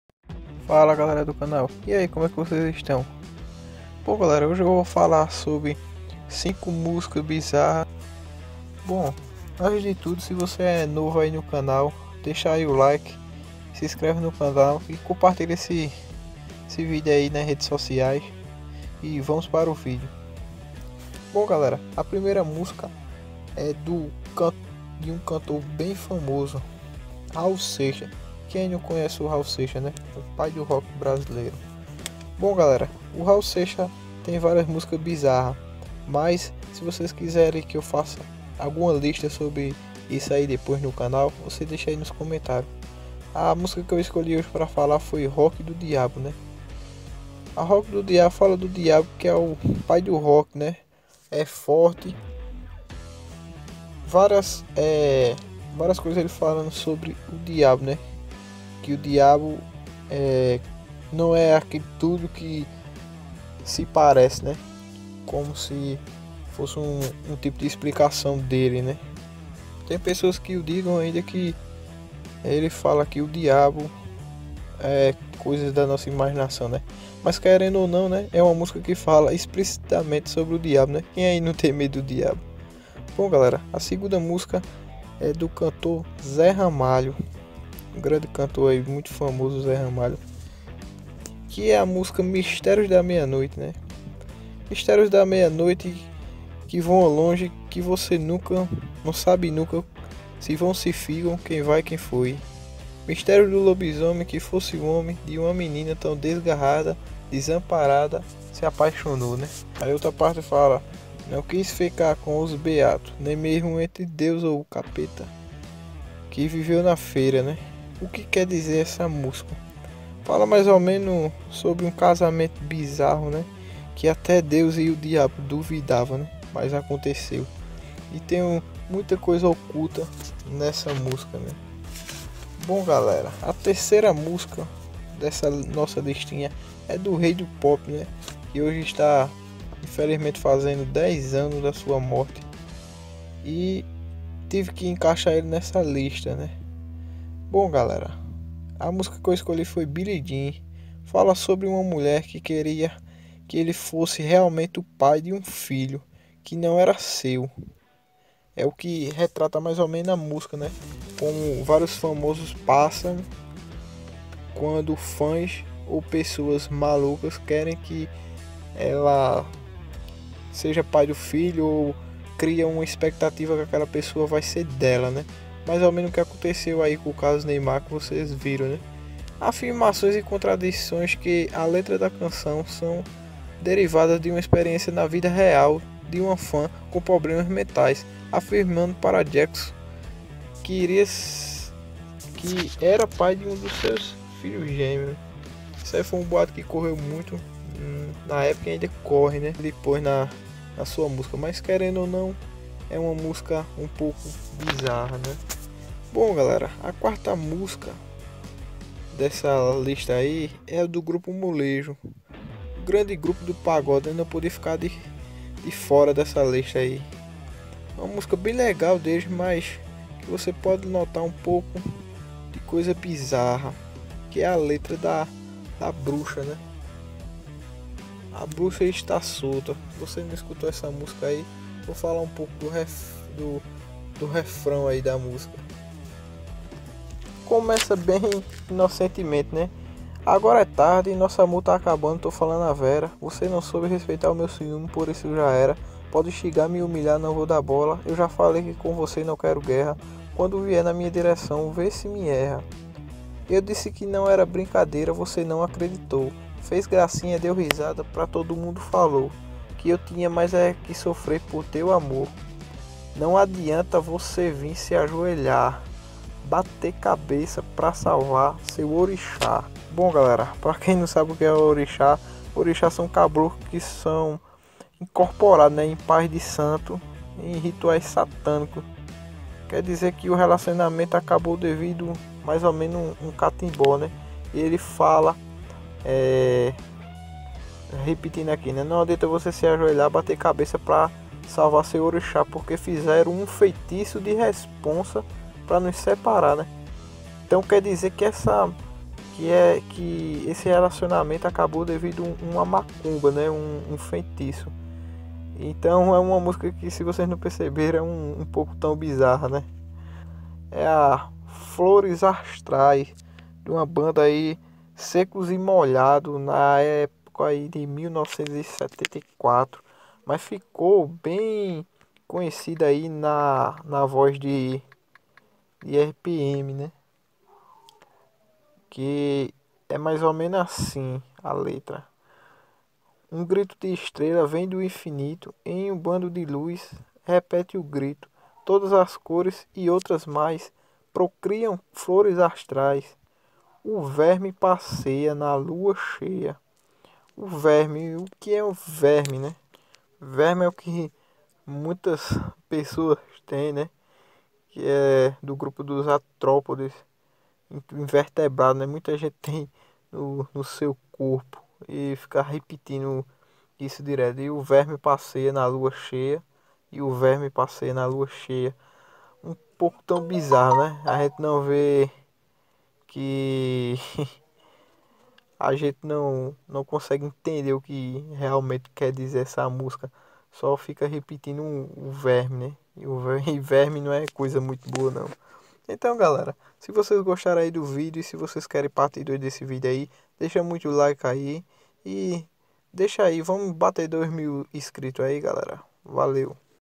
5 MUSICAS FAMOSAS QUE SÃO BIZARRAS QUE VOCÊ NUNCA REPAROU
fala galera do canal musicas bizarras que parecem inocentes de famosos e acho que talvez vocês nunca prestaram atenção